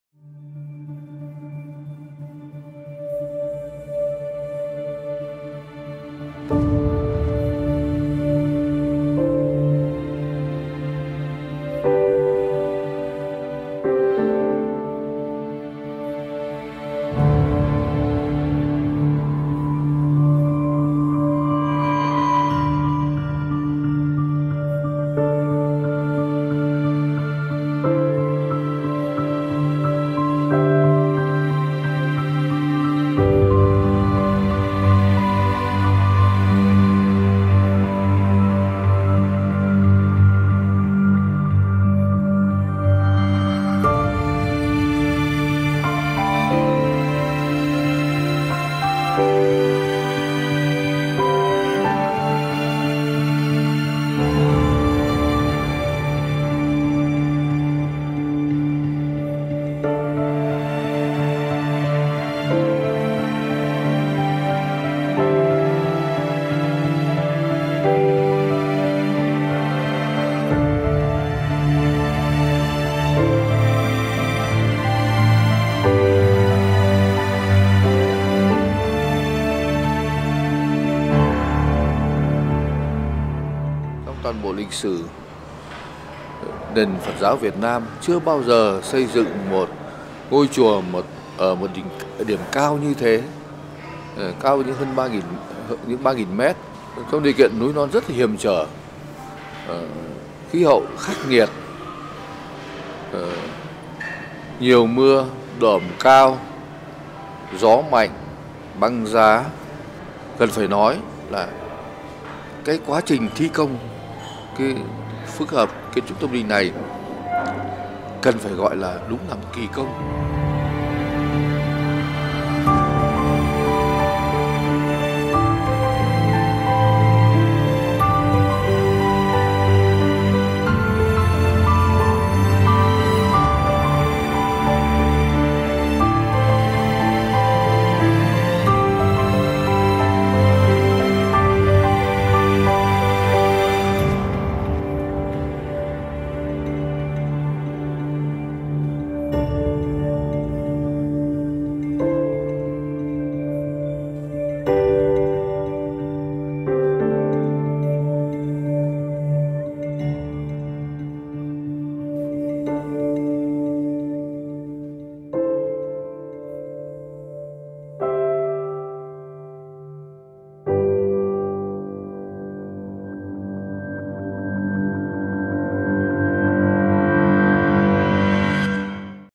Đầu xuân chiêm bái những quần thể tâm linh kỳ vĩ
Ở hai miền đất được mệnh danh là “đỉnh trời” của Việt Nam, nép trong mây núi, những công trình tâm linh kỳ vĩ giữa non ngàn là điểm hẹn của Phật tử và du khách bốn phương mỗi dịp Tết đến, xuân về…